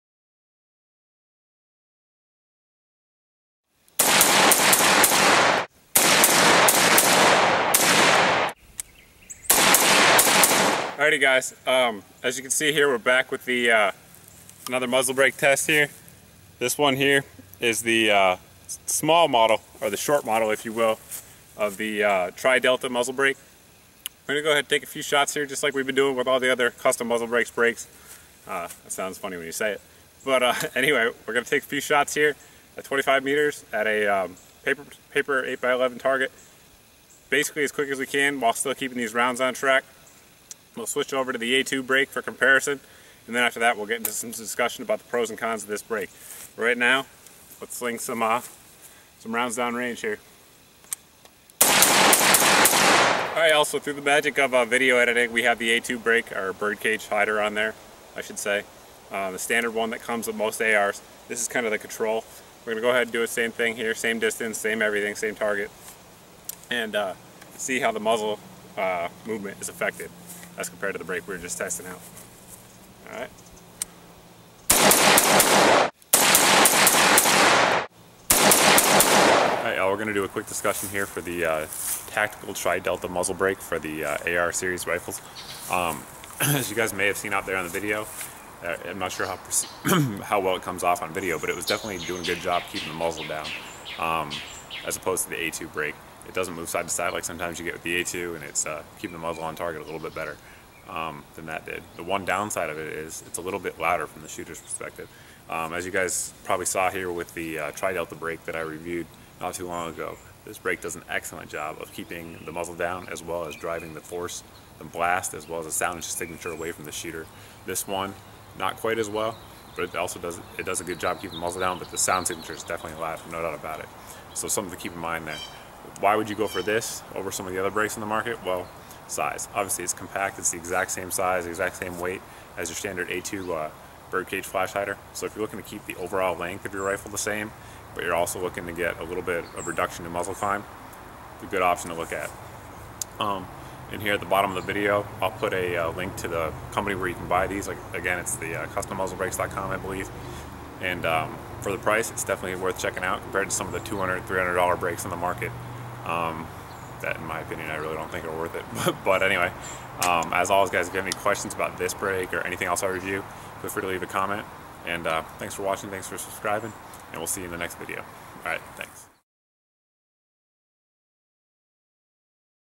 TriDelta Tactical AR Muzzle Brake From Custommuzzlebrakes Review
Test & Review of the muzzle brake.